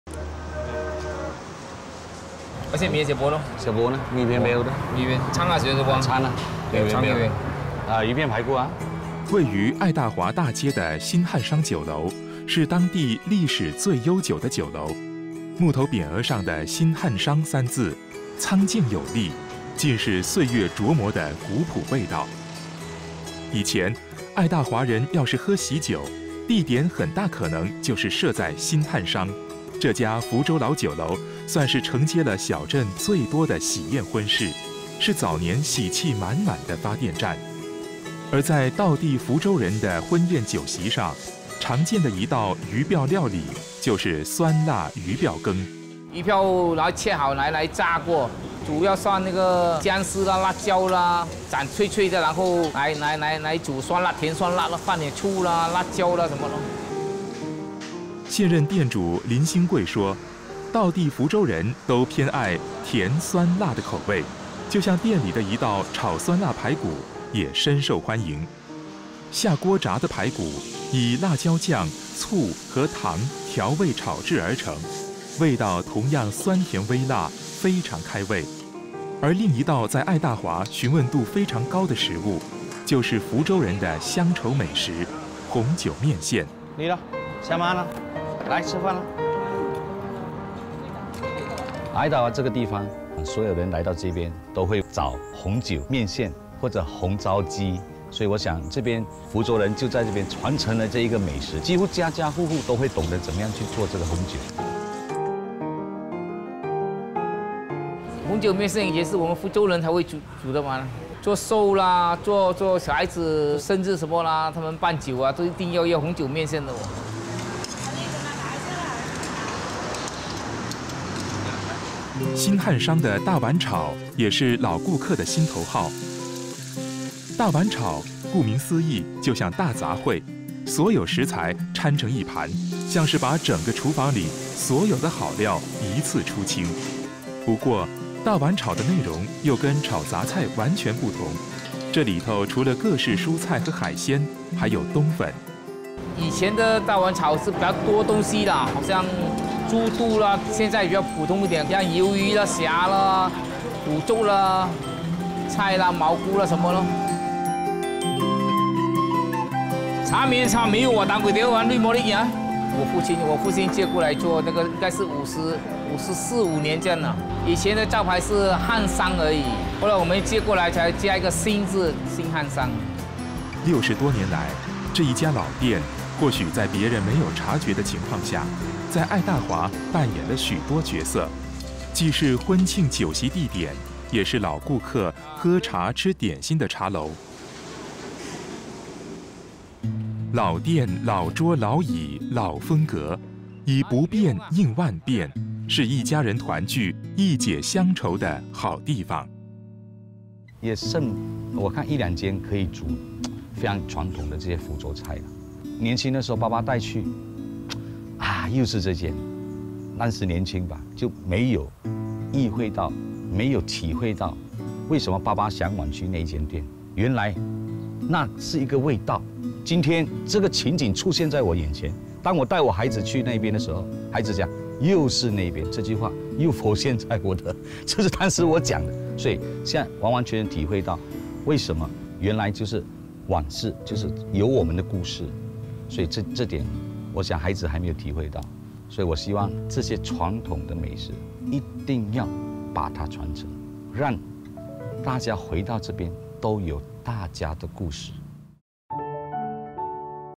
福州人甜酸辣滋味 Fuzhou Traditional Dishes
外頭美好的滋味，或許一口咬下時便能知曉；但鄉愁綿長的滋味，卻是要在歲月的慢慢淘洗中，才能品嚐得出來。食物，伴隨著人成長，當時或許不知道它的好，等到年輕不再，離開了又回來，才發現食物和人，也存有一種守候等待的無字約定。

老店老桌老椅老風格，以不變應萬變，是一家人團聚一解鄉愁的好地方。道地福州人都偏愛甜酸辣的口味，就像店裡的炒酸辣排骨、酸辣魚鰾都深受歡迎。

新漢商酒樓 Restoran Sun Hon Siong
142, Jalan Besar, 32400 Ayer Tawar, Perak.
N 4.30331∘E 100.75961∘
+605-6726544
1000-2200